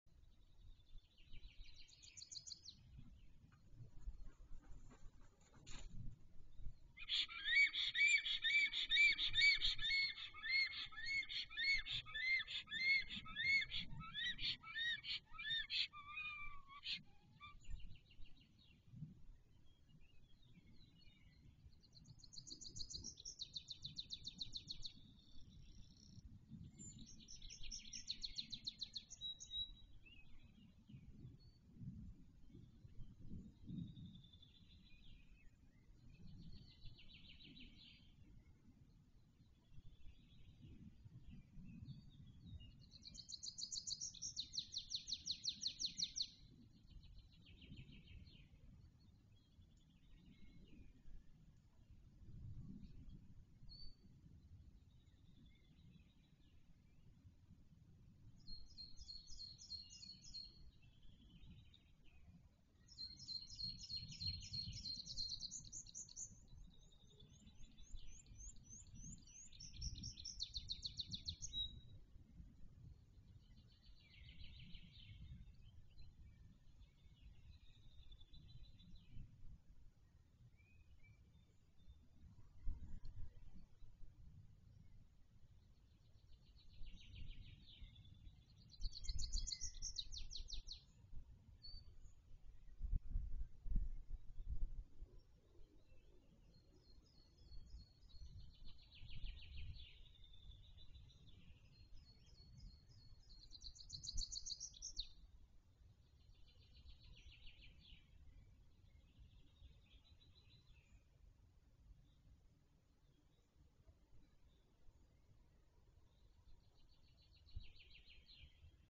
Późny powrót Tytana z afrykańskiej odysei / Titan's Late Return from the African Odyssey
Nie wiemy czemu tak późno i co zatrzymało tego bociana podczas wędrówki. Ptak zachowuje się swobodnie i widać, że zna to gniazdo. Najbliższe dni pokażą czy zacznie budować i czy znajdzie partnerkę. Jeszcze jest szansa na późny lęg :-)
We do not know why it is so late and what stopped this stork during its journey. The bird behaves freely and you can see that it knows the nest. The coming days will show whether he will start building and whether he will find a partner. There is still a chance for late breeding :-)
BY ANDER